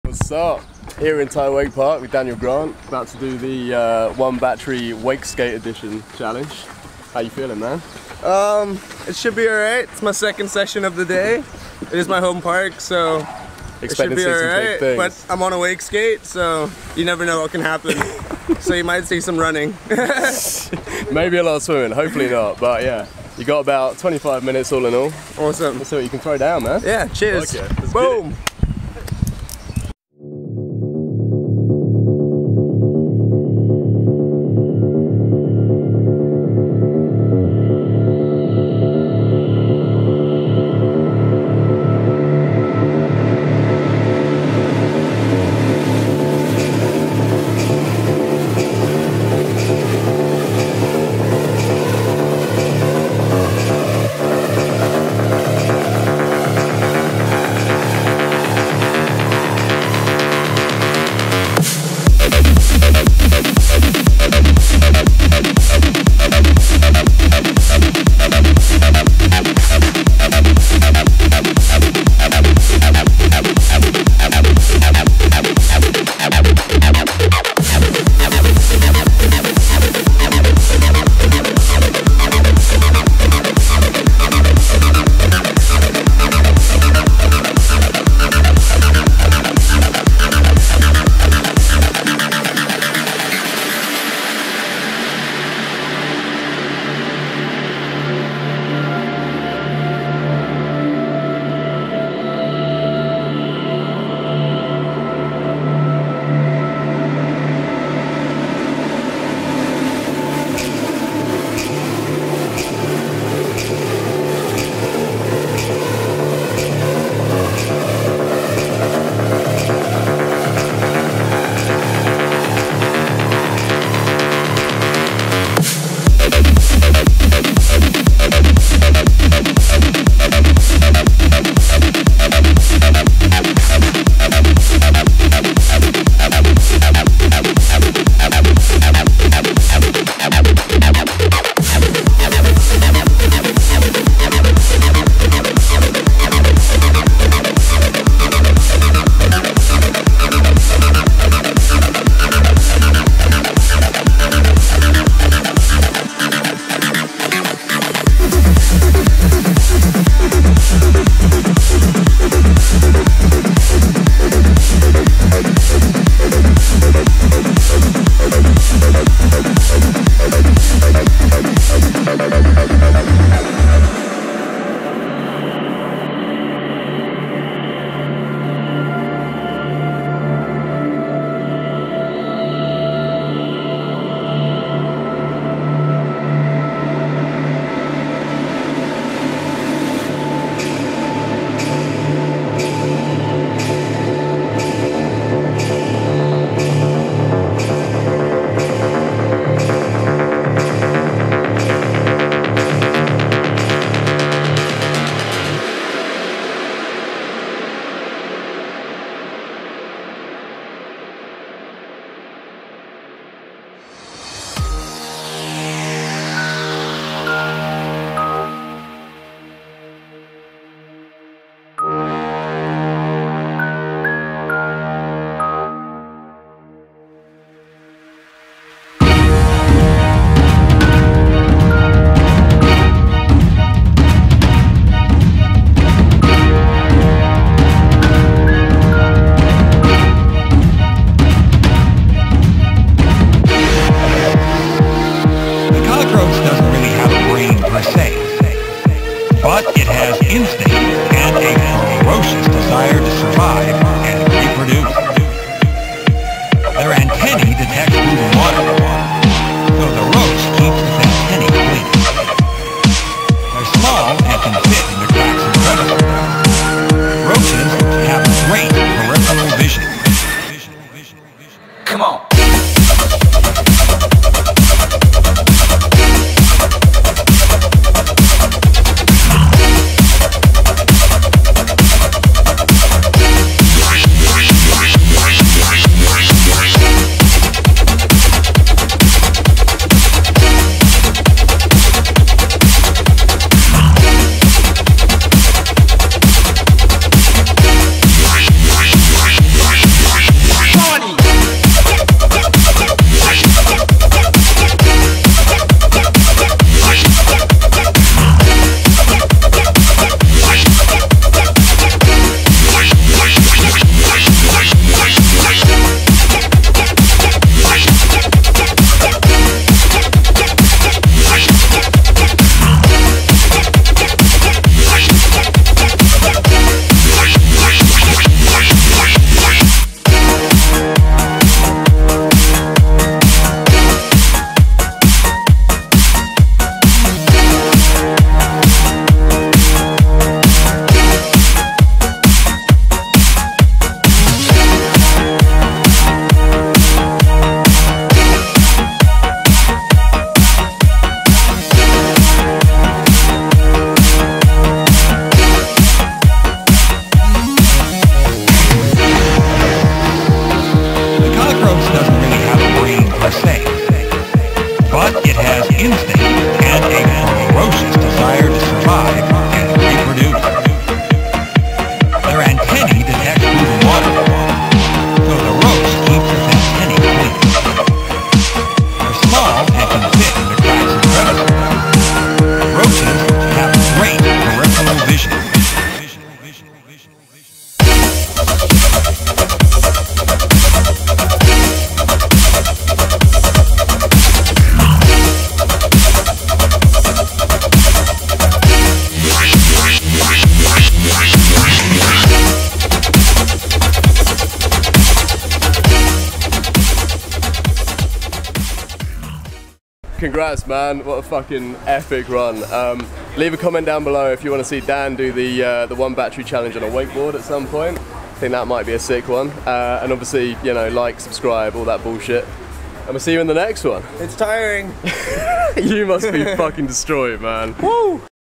Daniel Grant - One Battery Wakeskate @ Thai Wake Park
One battery wakeskate edition with Daniel Grant! What a machine...